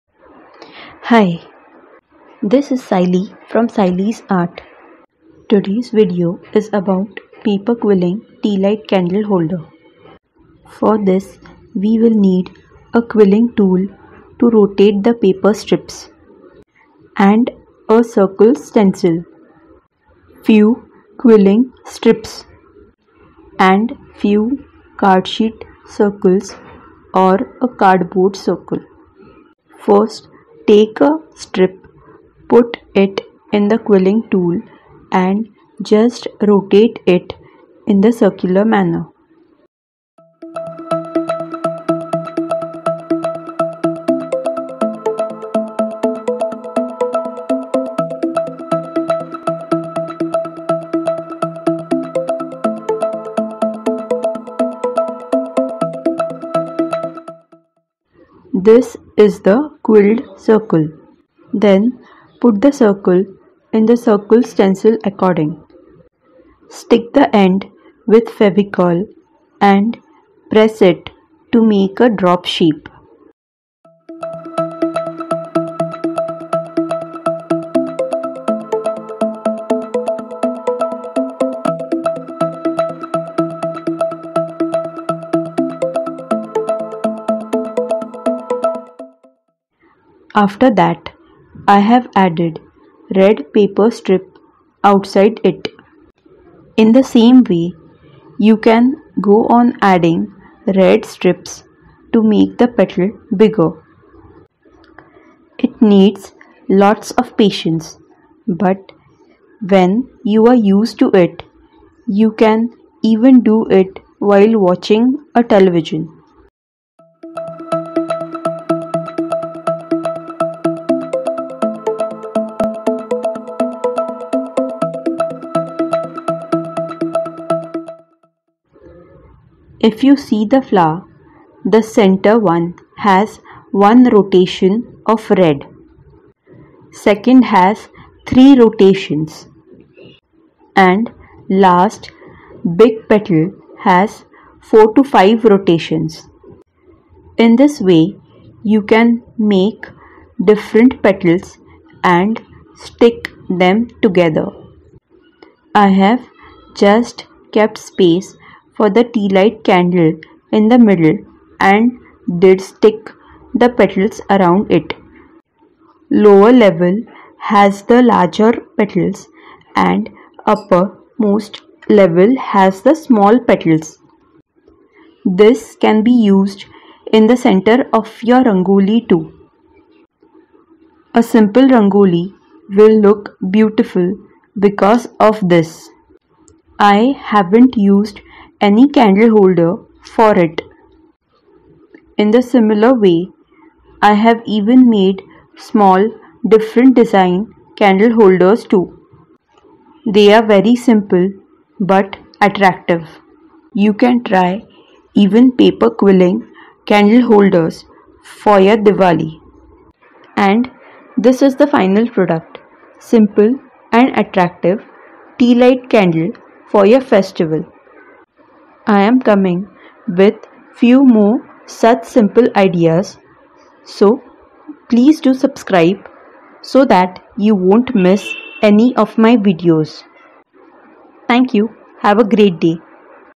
Paper quilling tea light candle holder- 7
Its about tea light candle decoration. festival time we need such decorative items at our houses which we can prepare ourselves. This is made with paper quilling strips which we get easily at the store.

material needed
1.Quilling strips 5mm
2.tea light candle
3.cardsheet or cardboard(radius 6cm)
4. fevicol